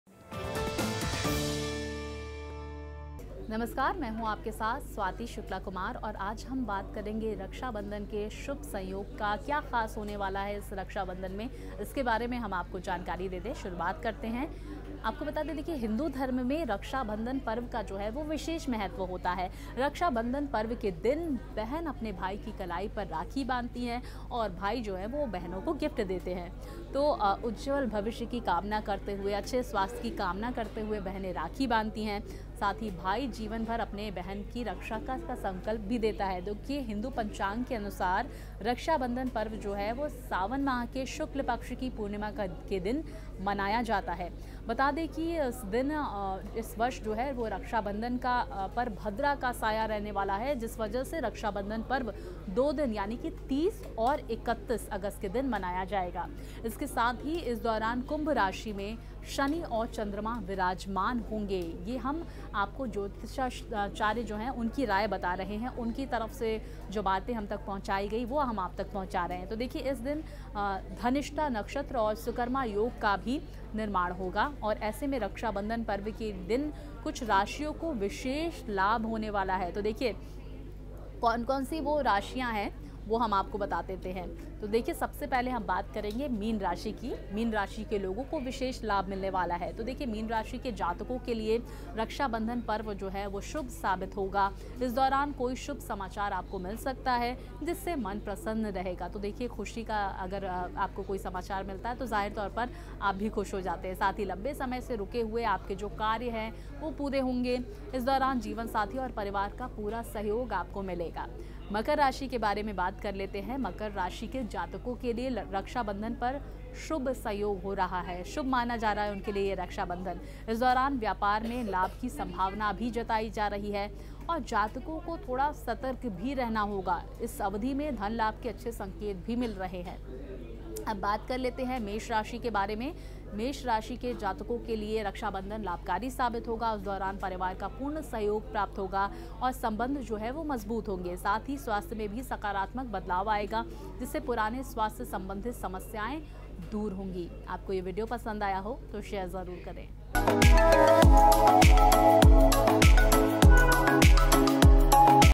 Raksha Bandhan 2023: Raksha Bandhan पर हो रहा है अत्यंत शुभ योग का निर्माण, इन राशियों के लिए है शुभ
Raksha Bandhan 2023: Raksha Bandhan पर हो रहा है अत्यंत शुभ योग का निर्माण, इन राशियों को मिलेगा भाग्य का भरपूर साथ